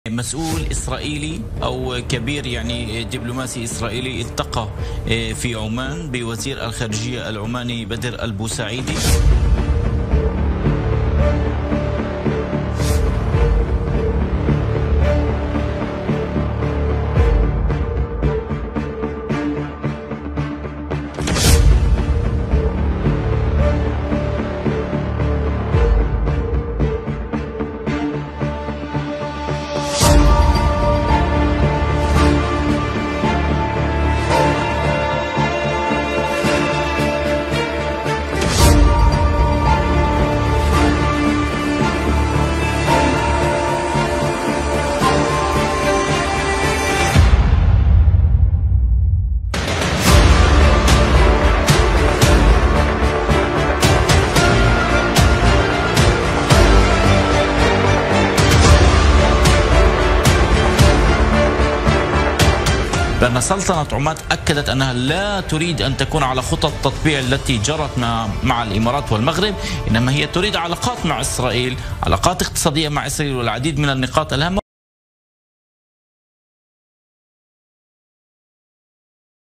عُمان في ركب التطبيع؟
محاولات التطبيع من جديد..

لقاءٌ يجمع مسؤولين "إسرائيليين" مع وزير خارجية سلطة عُمان!